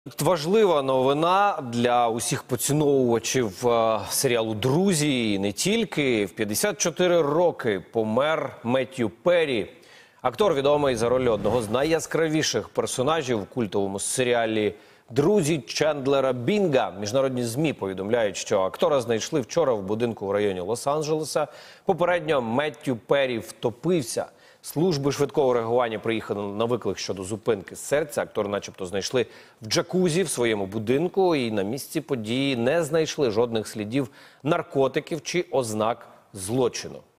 Помер зірка серіалу "Друзі" Меттью Перрі
У 54 роки помер зірка серіалу "Друзі" Меттью Перрі. Тіло Меттью Перрі знайшли в суботу в його будинку в районі Лос-Анджелеса.